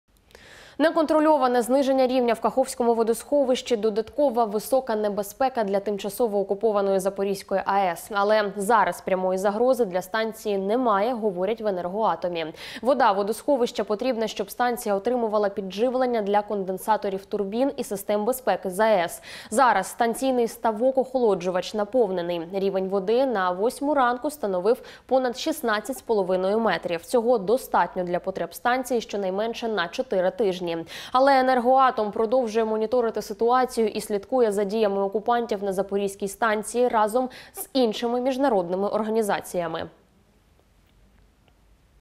“Наразі прямої загрози для Запорізької АЕС немає”, - повідомляють в Енергоатомі
Наразі прямої загрози для станції немає. Говорять в Енергоатомі. Вода водосховища потрібна, щоб станція отримувала підживлення для конденсаторів турбін і систем безпеки ЗАЕС. Зараз станційний ставок-охолоджувач наповнений. Рівень води на 8 ранку становив - понад 16,5 метрів. Цього достатньо для потреб станції щонайменше на 4 тижні.  _______________________________________________